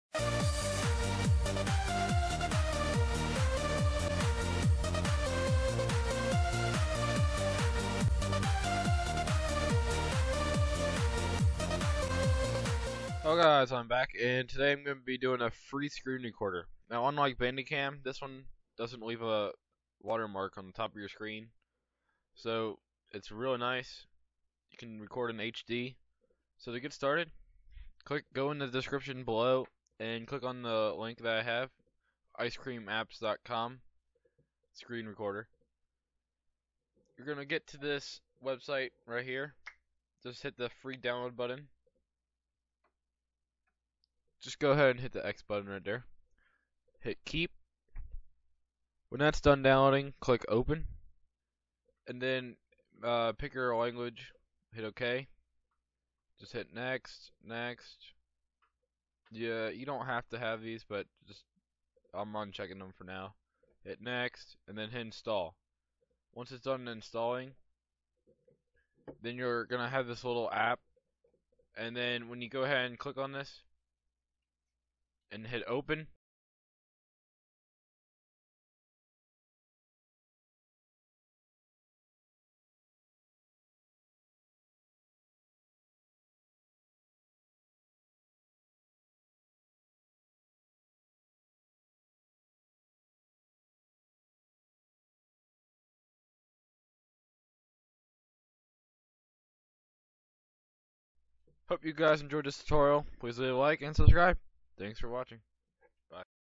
Icecream Screen Recorder Free
Icecream Screen Recorder is a free screen recorder that lets the user produce HD videos without a watermark unlike Bandicam. The setup is quick and easy. I show you how to download and install the program.

Don't want a screen recorder but want to check out their

Hope you liked this video if you did a like would be appreciated.